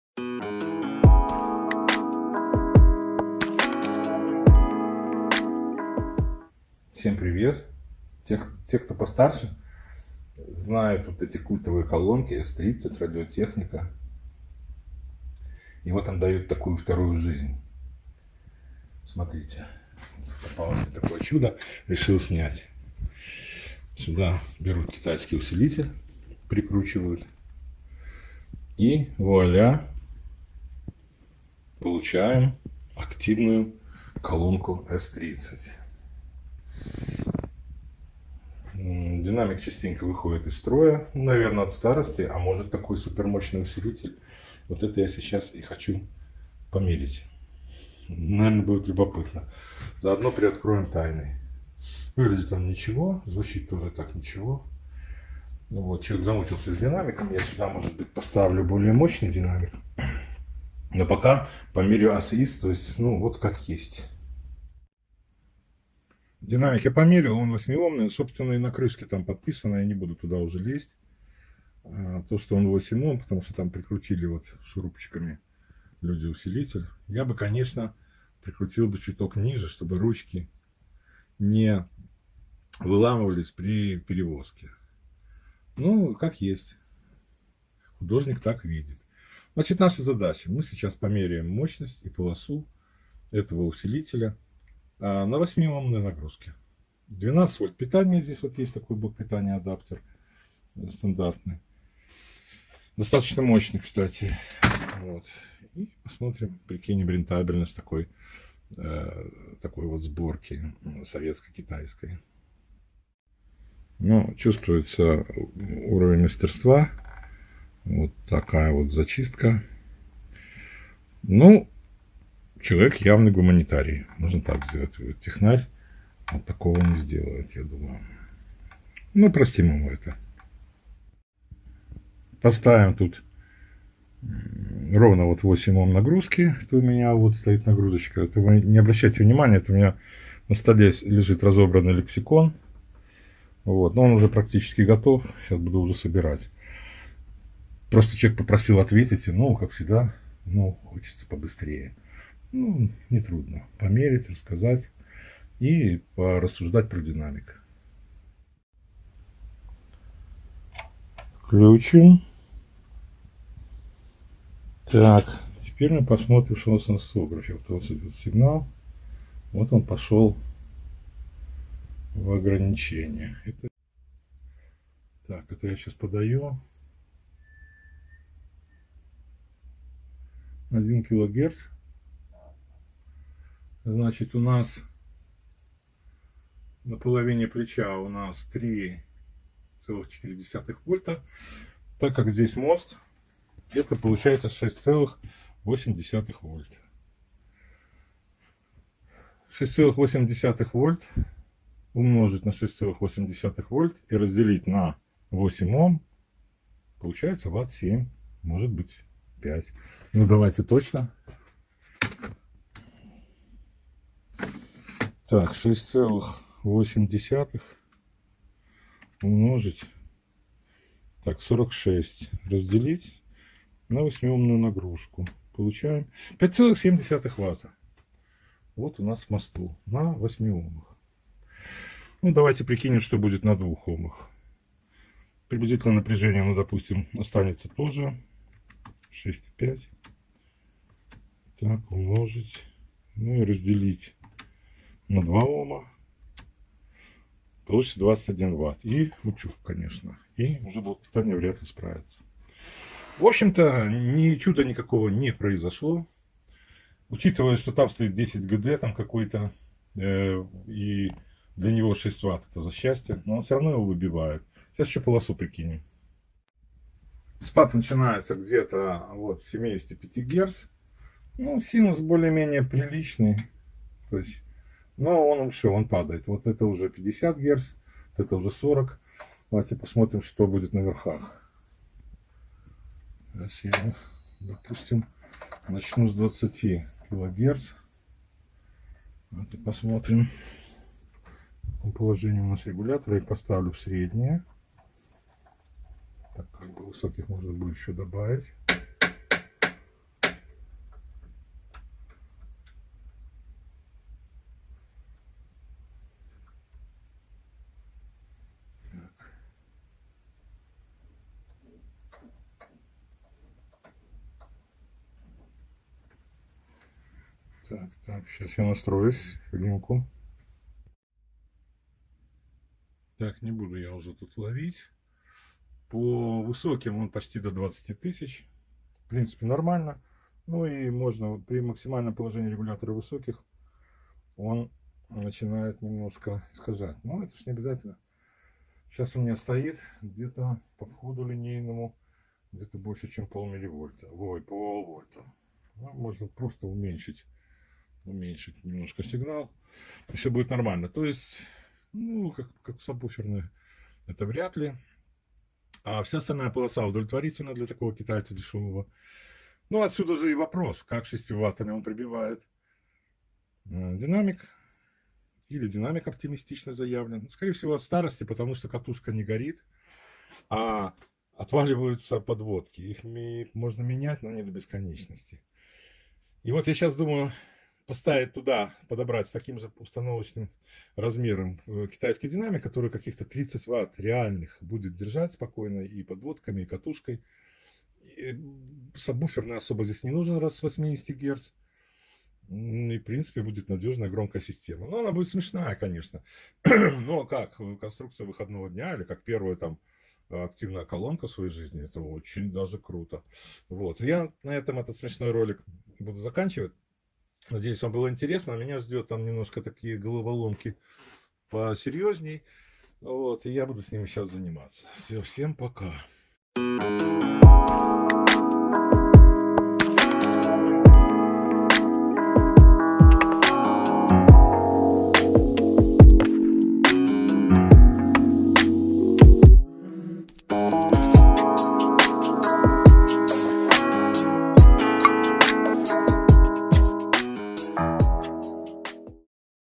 Неравный брак разница 42 года
42х летняя S-30 из СССР объединилась с годовалым китайчиком.Но он оказался молодцом только внешне :)